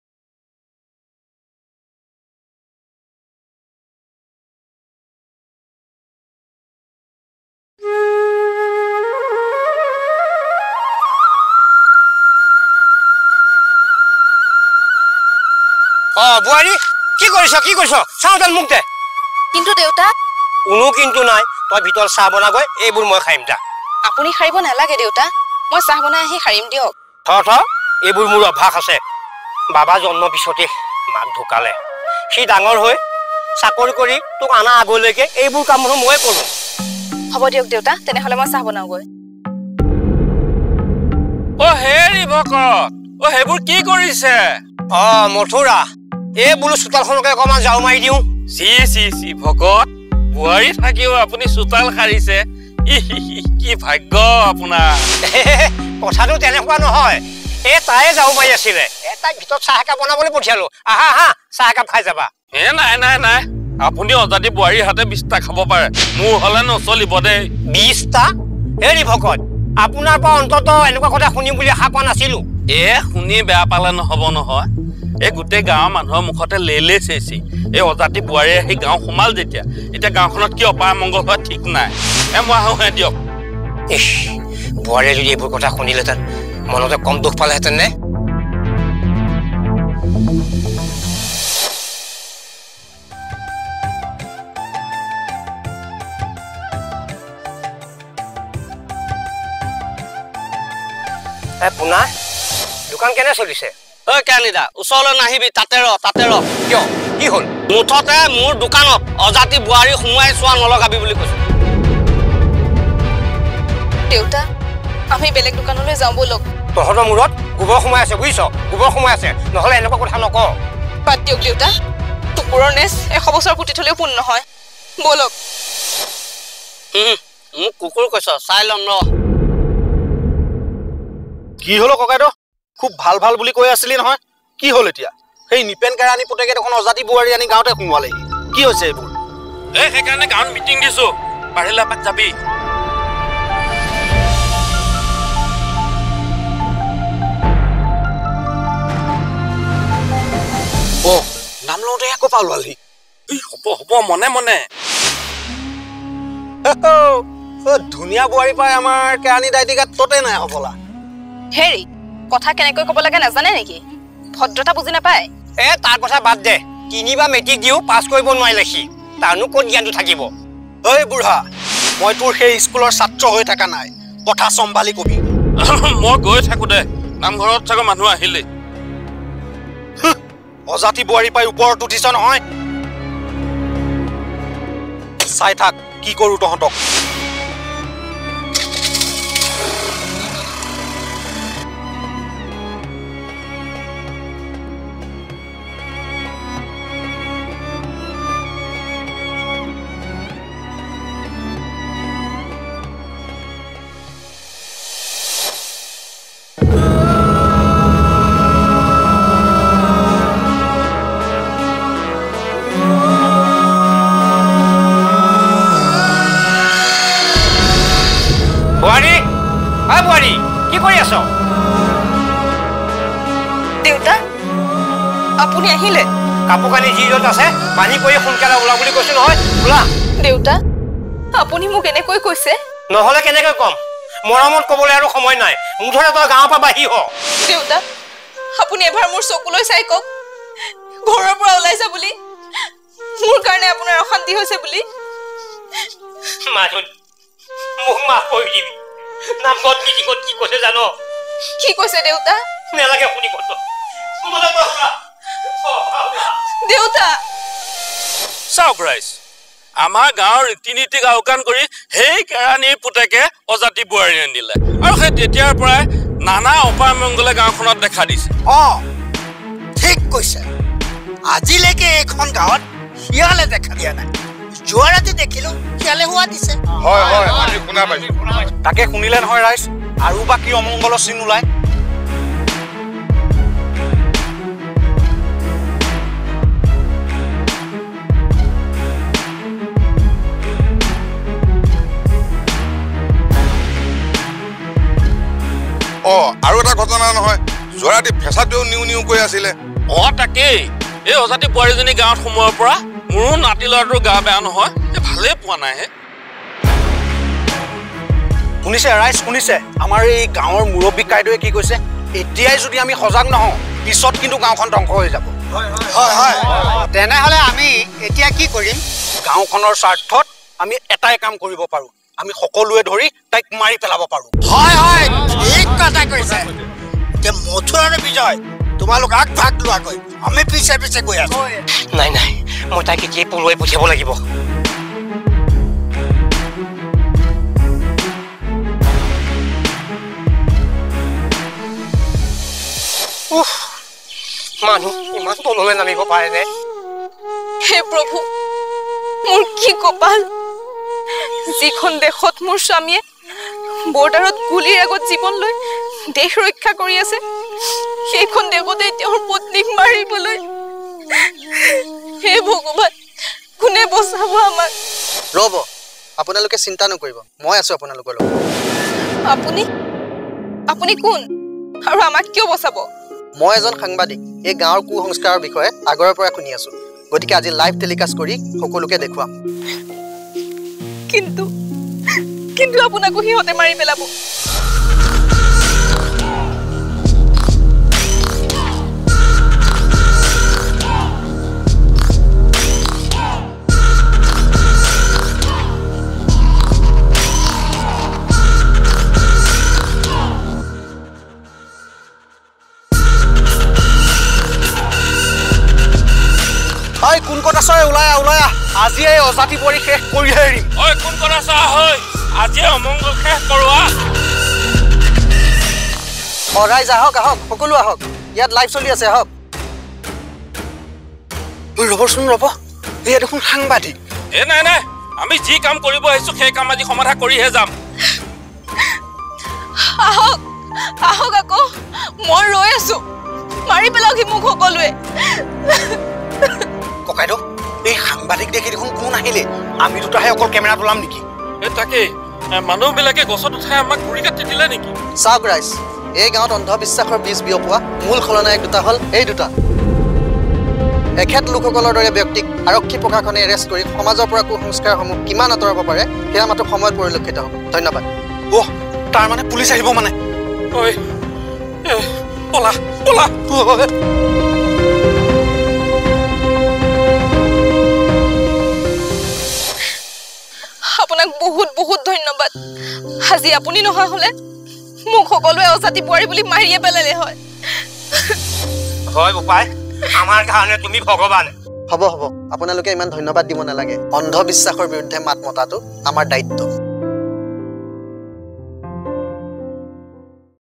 Ojati Buwari 3 - Assamese sad story - Assamese real story - UDP Entertainment
Ojati Buwari part 3 is a new Assamese sad story or Assamese heart touching video by UDP Entertainment